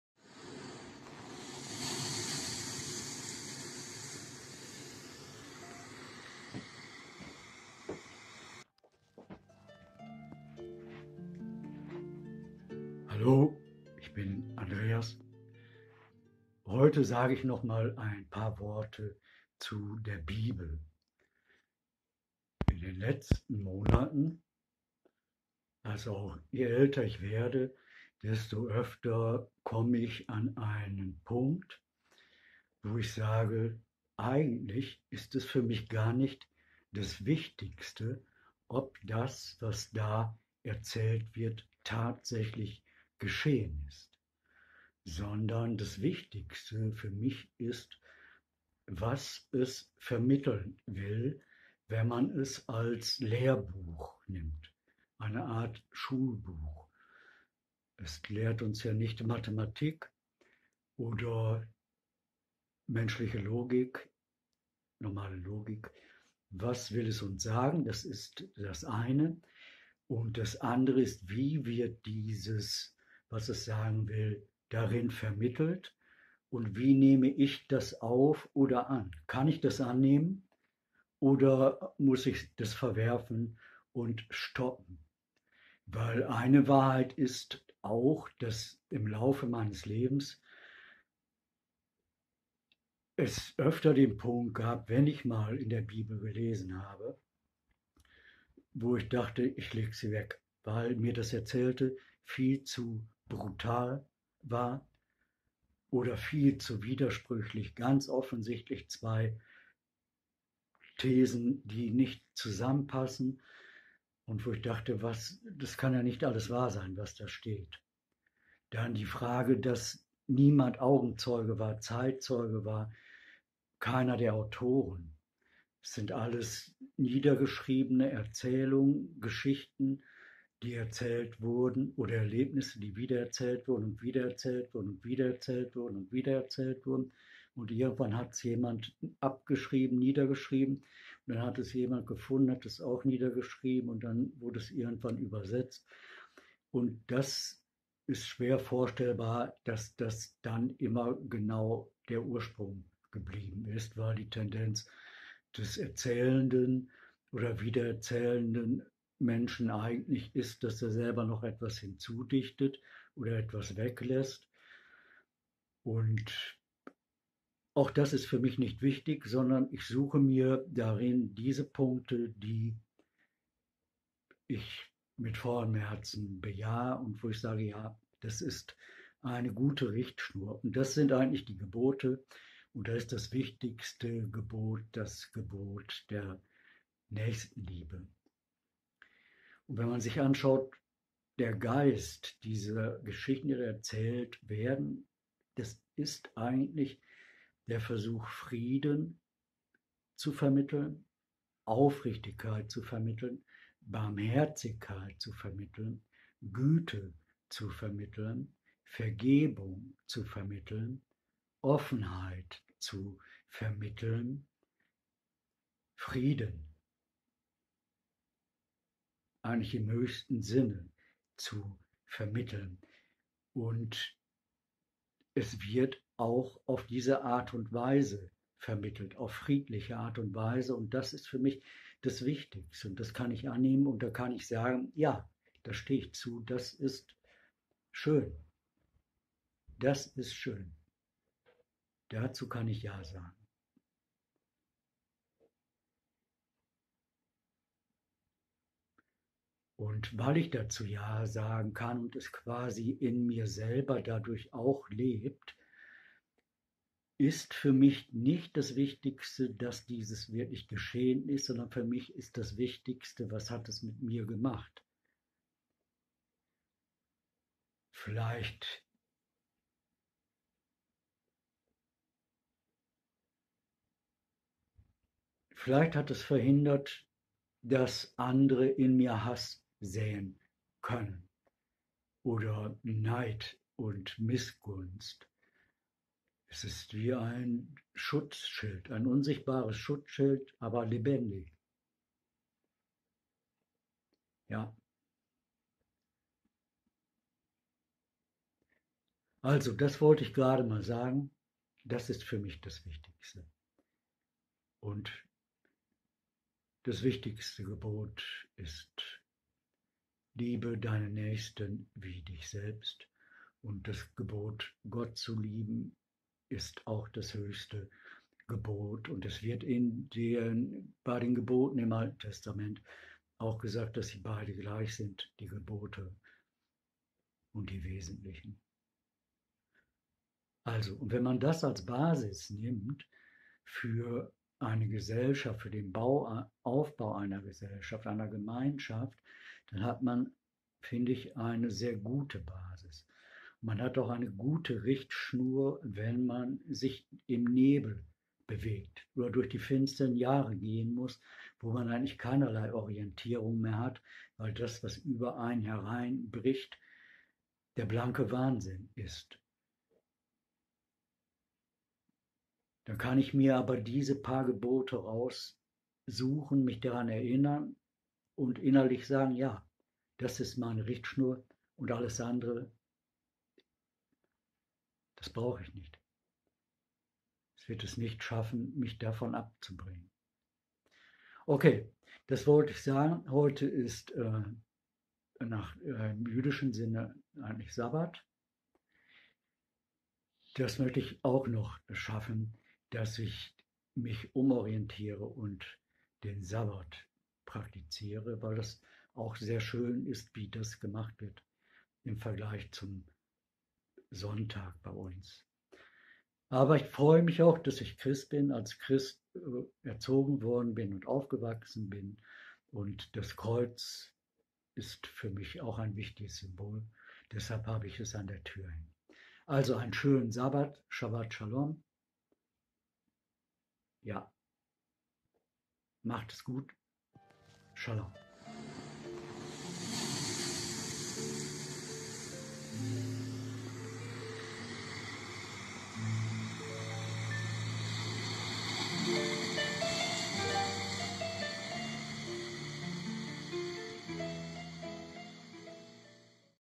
"Gedanken zur Bibel"
Was wesentlich für mich ist. Der Kern. Die Liebe. Die Liebe ist für mich das Göttliche, "Gott". Die Liebe zu Gott und die Nächstenliebe sind die beiden höchsten Gebote. Sie sind gleichwertig  /Which is essential for me. The core. Love. Love is for me the divine, "God". Love for God and charity are the two highest commandments. They are equivalent  💔